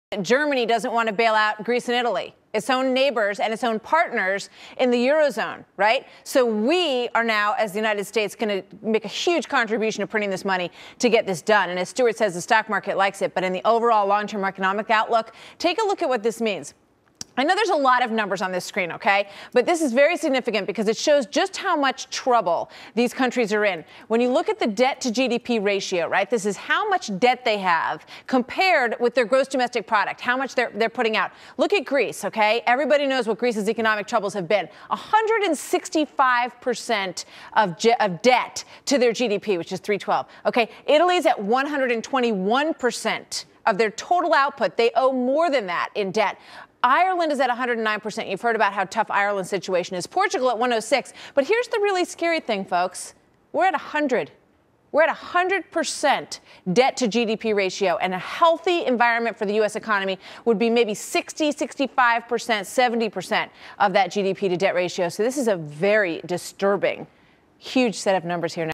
Eurozone debt vs US debt
How long can we borrow more than we have? No president in the history of the world has increased national debt by as much as Obama has in his first two and half years. And he still wants more spending. Maybe he really does want to collapse the economy.